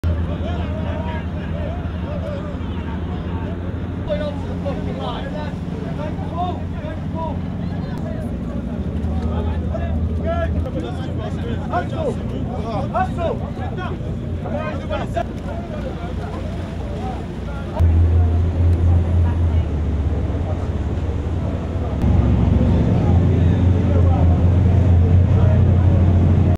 Royal Navy's latest rescue mission in the Mediterranean
They are working to bring more than 400 people to safety after they were found 40 miles from the Libyan coast in inflatable boats.